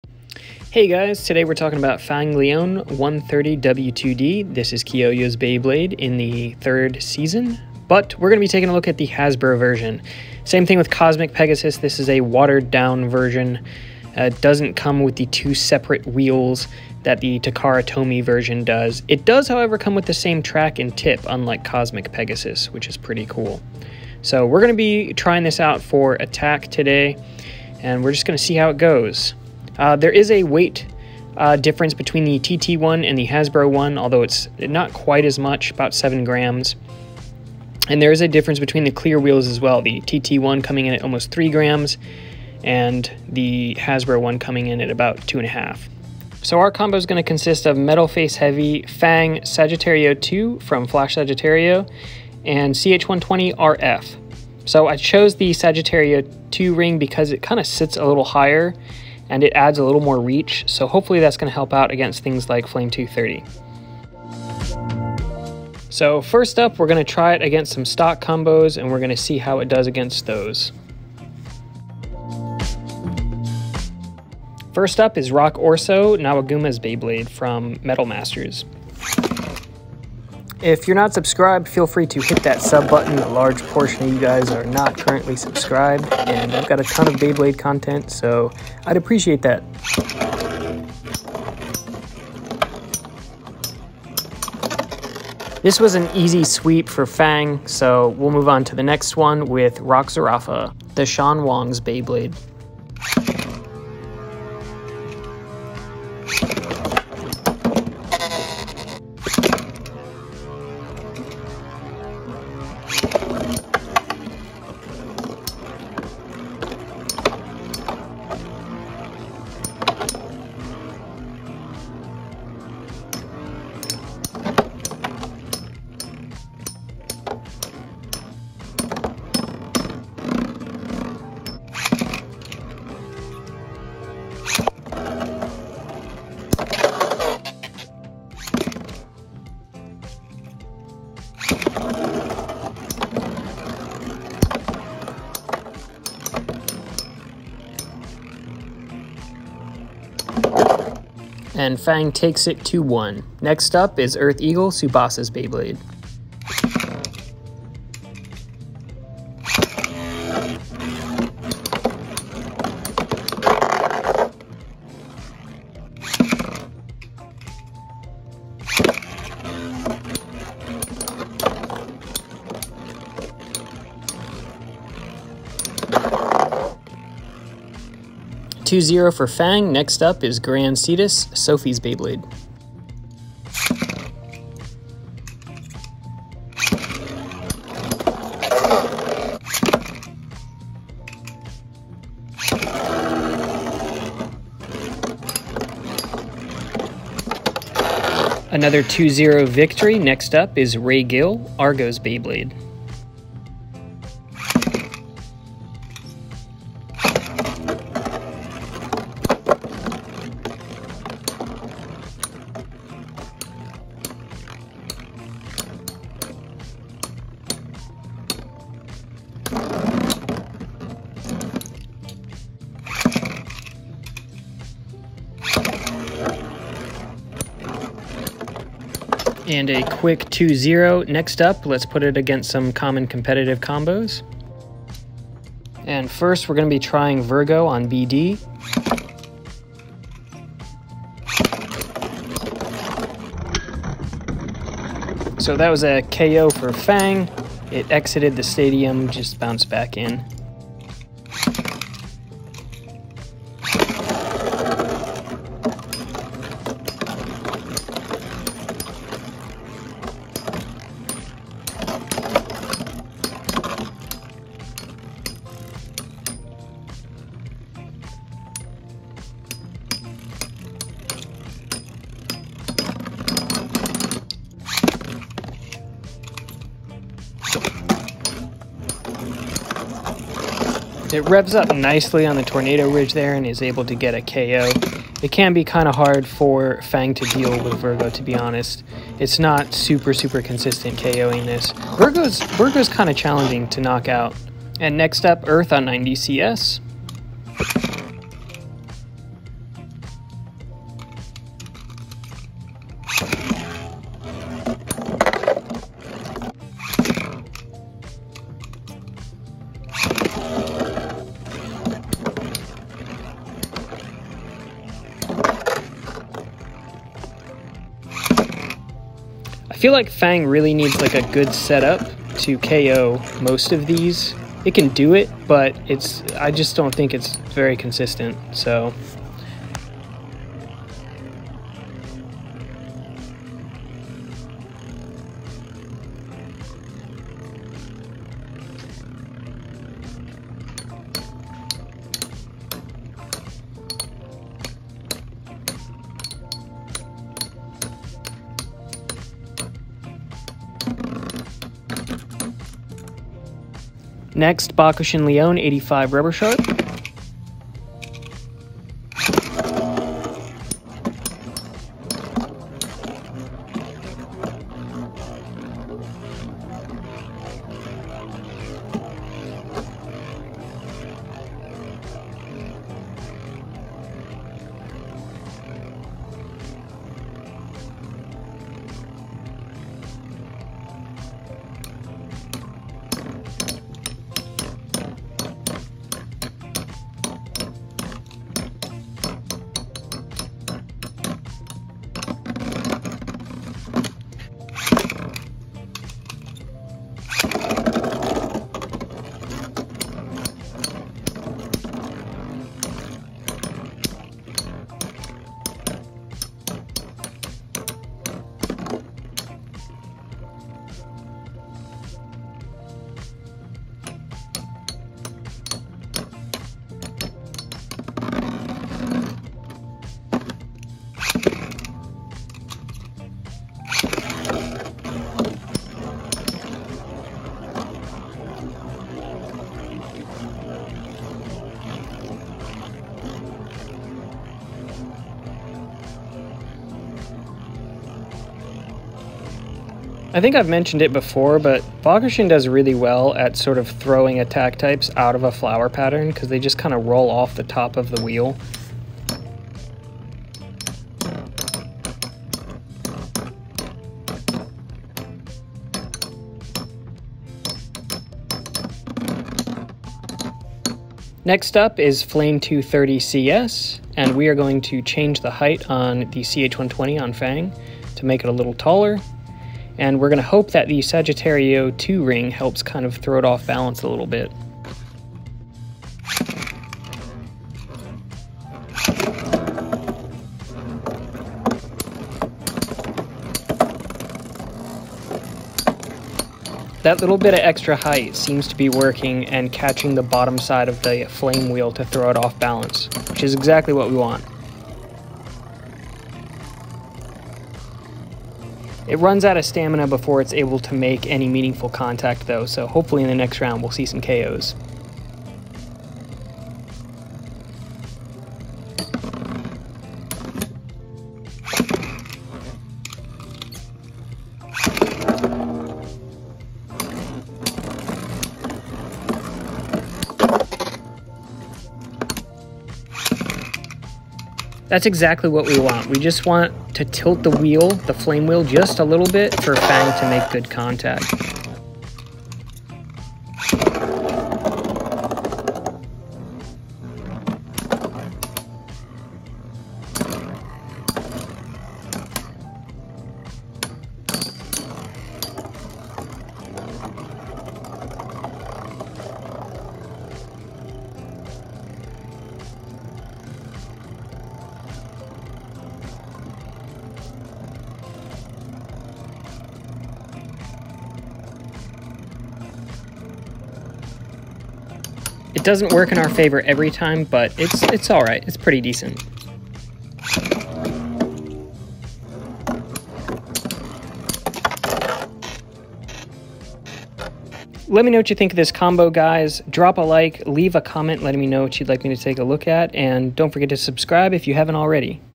What IF WE turned FANG LEONE into an ATTACK TYPE?
Is this the most underrated leone metal fight beyblade? At least fang leone comes with w2d, a really good tip for competitive use. Let me know what you think of this beyblade and this combo in the comments!

Intended audience 13+

Intended audience 13+